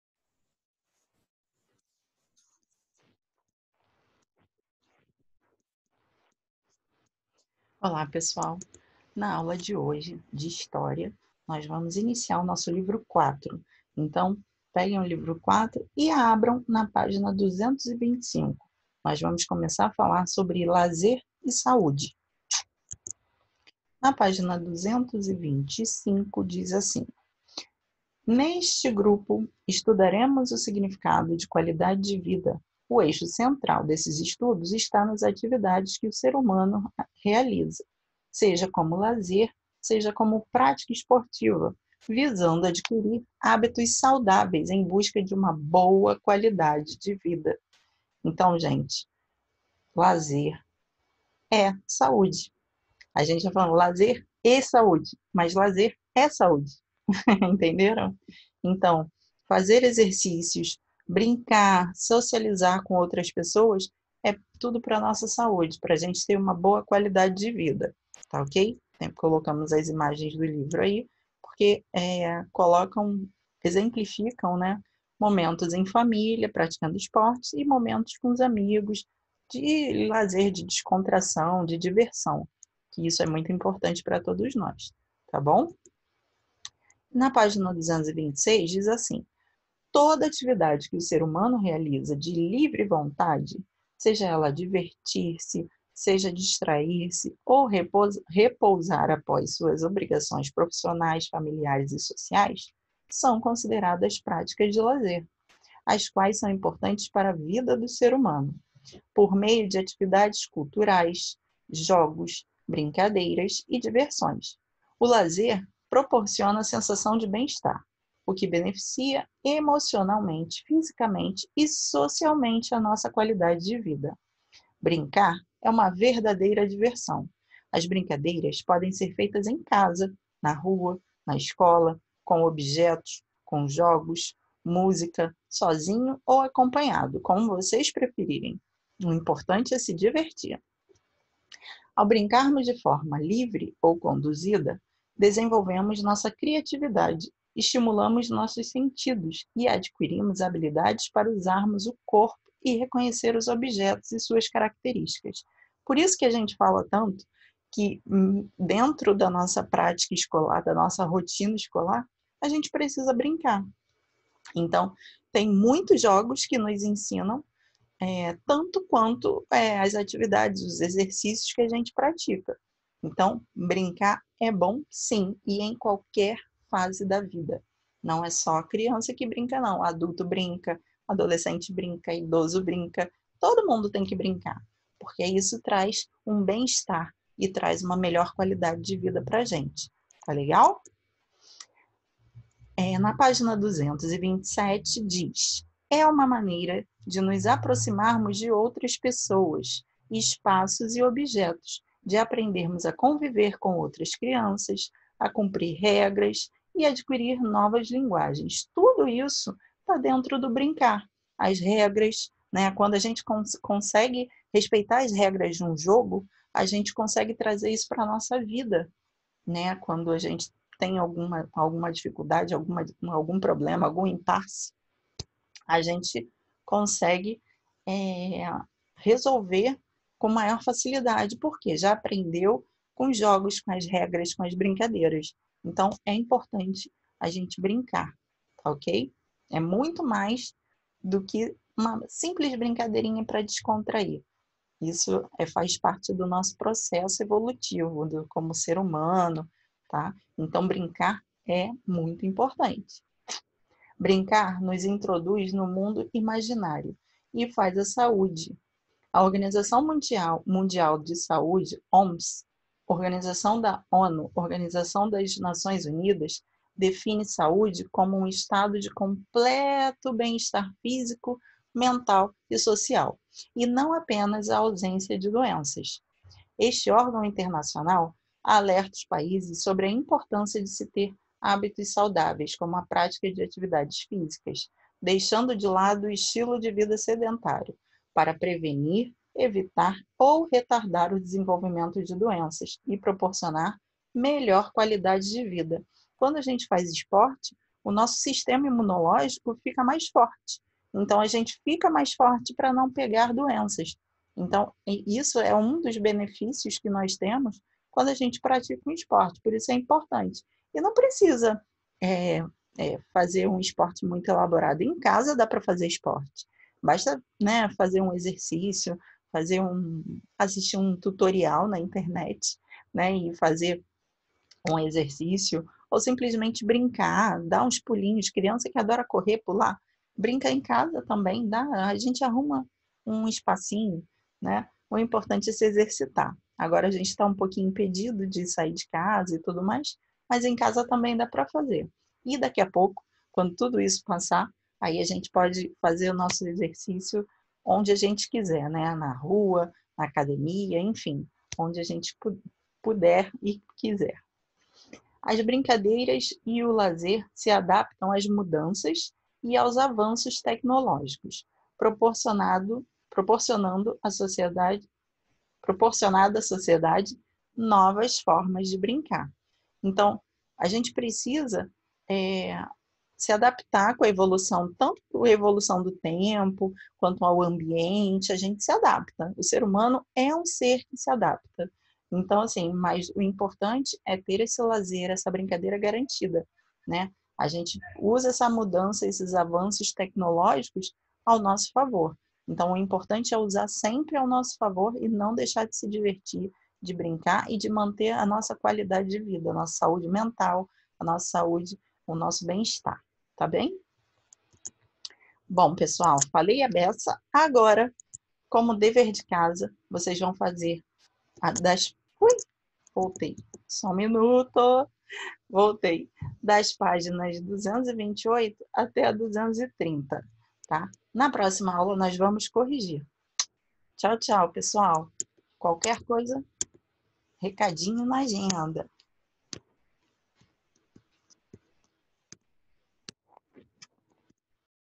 Aula 16 de História - 2º ano - Professora Vanessa
- MATRÍCULAS ABERTAS -

Aula 16 de História com a professora Vanessa, para o 2º ano. Essa aula vai te ajudar a manter a consistência que você precisa nos seus estudos.